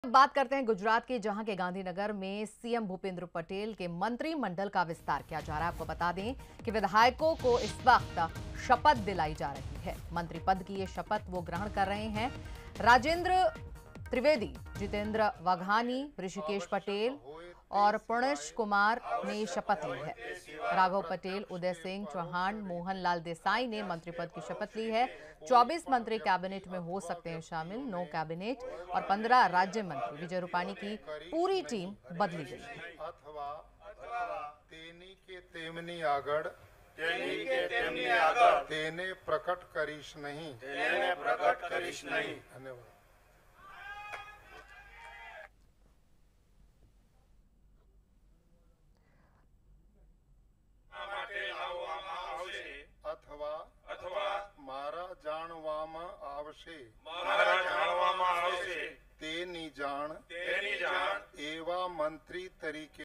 CM भूपेंद्र पटेल मंत्रिमंडल गठन। 24 मंत्री कैबिनेट में शामिल। विजय रुपाणी की टीम बाहर
Video credit- @ANI
गुजरात के नए मुख्यमंत्री भूपेंद्र पटेल ने अपनी पूरी नई टीम बना ली है। राजभवन में हुए शपथ ग्रहण में 24 विधायकों को मंत्री पद की शपथ दिलाई गई। भूपेंद्र मंत्रिमंडल में 10 कैबिनेट और 14 राज्य मंत्री हैं। पूर्व मुख्यमंत्री विजय रुपाणी की 22 मंत्रियों वाली पूरी टीम बाहर हो गई है, इनमें पूर्व उप-मुख्यमंत्री नितिन पटेल भी शामिल हैं।